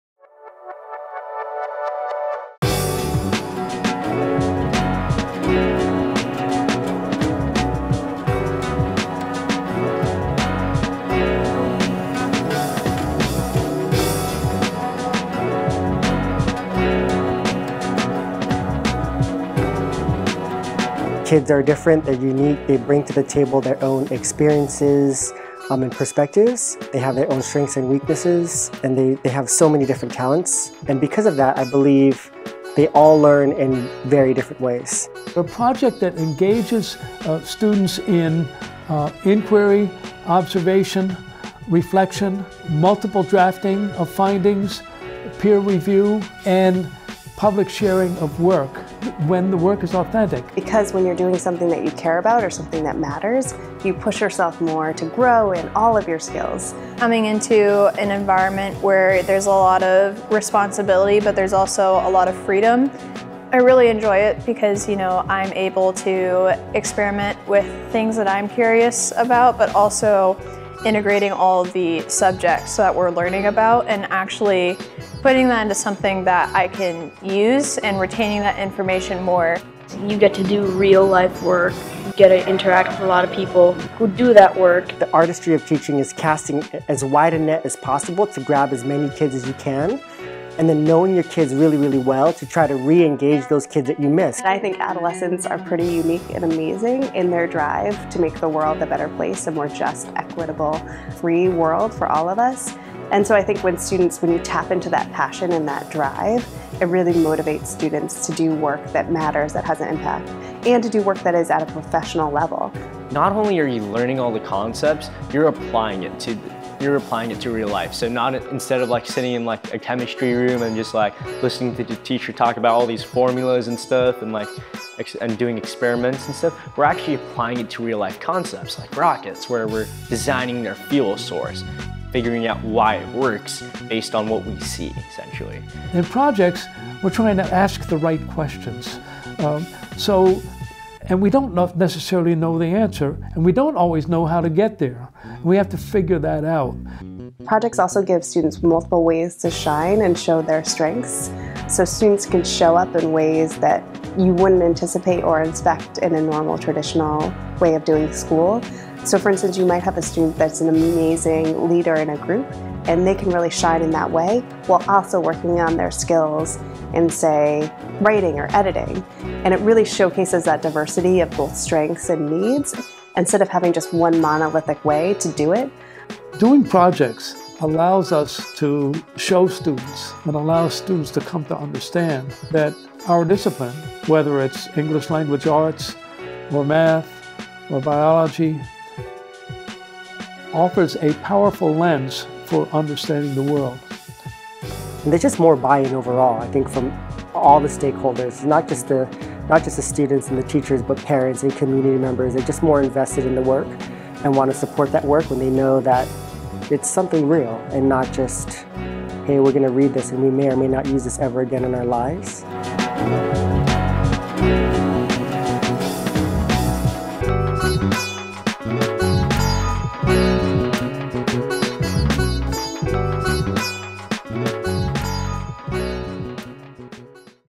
Why PBL?
High Tech High students, teachers and directors explain the thinking behind doing projects.